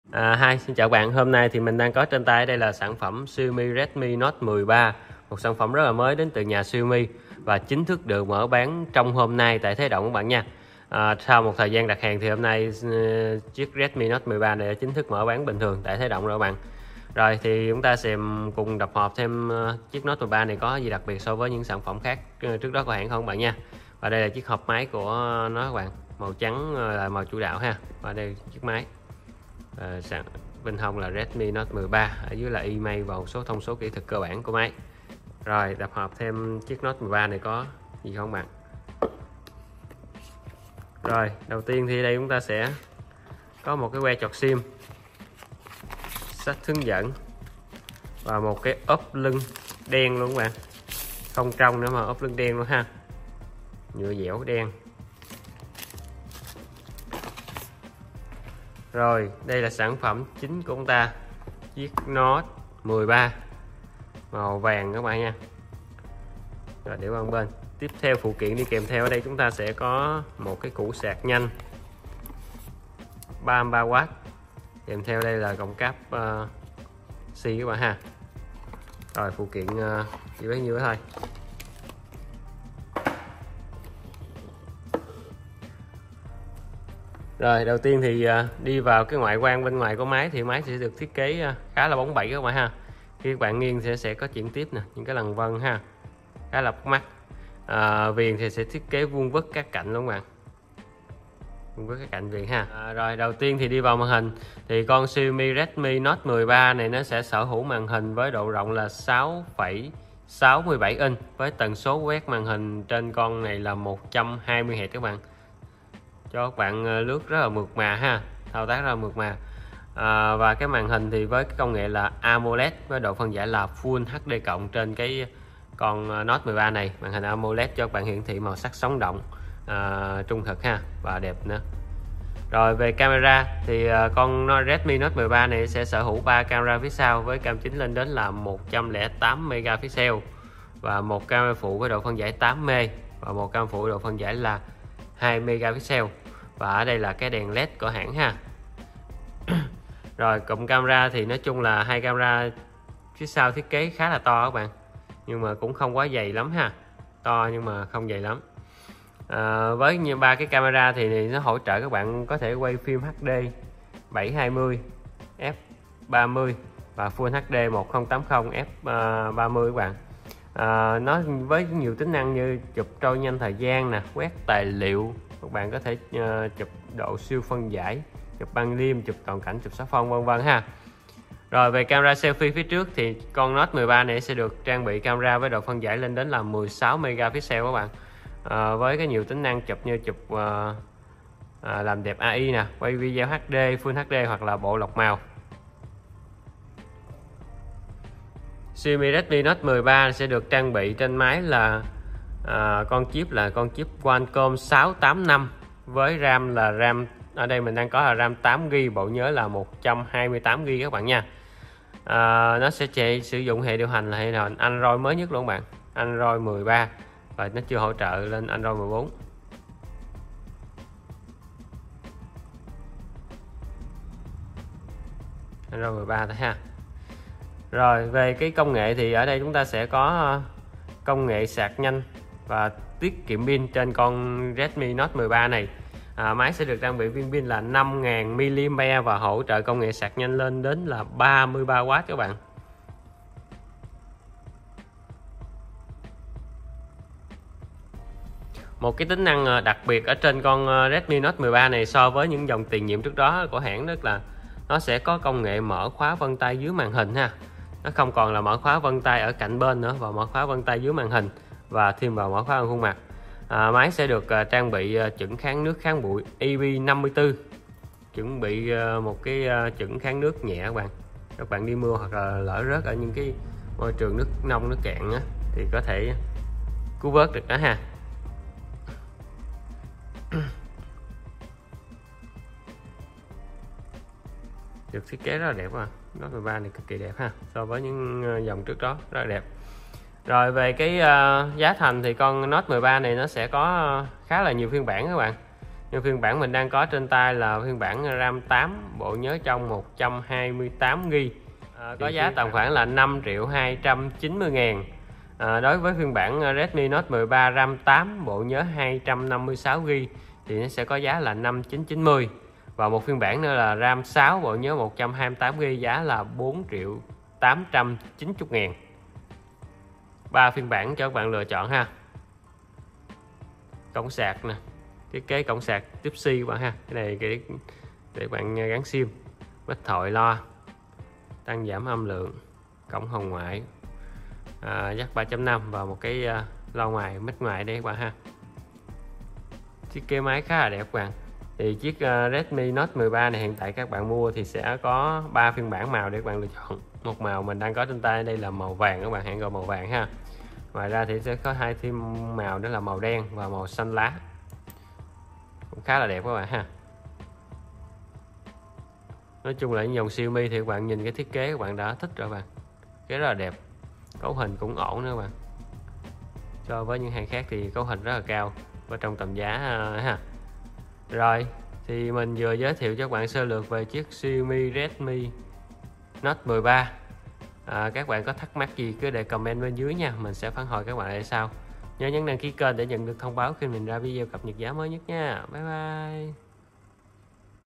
Xiaomi Redmi Note 13 : Mở Hộp Redmi Note 13 Máy Đẹp, Cấu Hình Ngon, Giá Rẻ tại Thế Giới Di Động
Màn hình: AMOLED 6.67" Full HD+
Chip: Snapdragon 685 8 nhân
RAM: 6 GB
Dung lượng: 128 GB
Camera sau: Chính 108 MP & Phụ 8 MP, 2 MP
Pin, Sạc: 5000 mAh 33 W